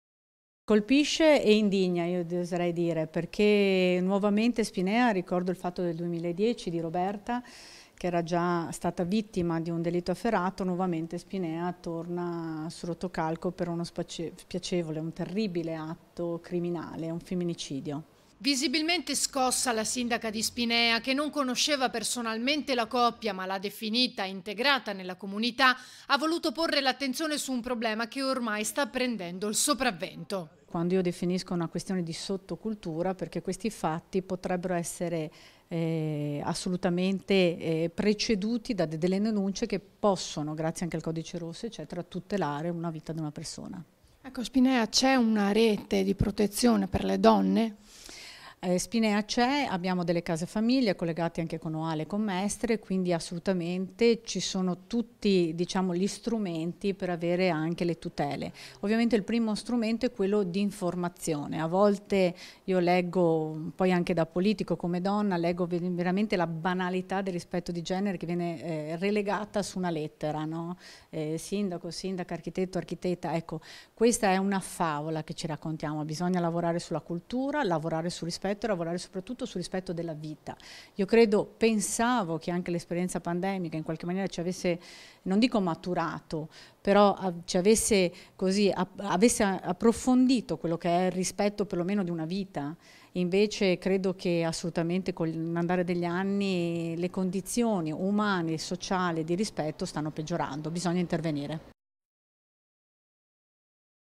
OMICIDIO SPINEA, IL SINDACO: «SI STA PEGGIORANDO, BISOGNA INTERVENIRE» | 23/09/2022
A3 NEWS Veneto 23/09/2022 - SPINEA - “E’ un problema di sottocultura, le condizioni stanno peggiorando e bisogna intervenire”. Lo pensa il sindaco di Spinea, Martina Vesnaver.  || Visibilmente scossa, la sindaca di Spinea martina Vesnaver, che non conosceva personalmente la coppia ma l’ha definita integrata nella comunità, ha voluto porre l’attenzione su un problema che ormai sta prendendo il sopravvento. "Si tratta di una questione di sottocultura, ha detto, la situazione sta peggiorando e bisogna intervenire in fretta". Con tutta probabilità, una volta chiarita la situazione, il comune aiuterà anche il piccolo rimasto solo. - Intervistati MARTINA VESNAVER (SINDACO DI SPINEA) (Servizio di Ilaria Marchiori) - Segui Antenna Tre anche sul digitale terrestre! Visita il sito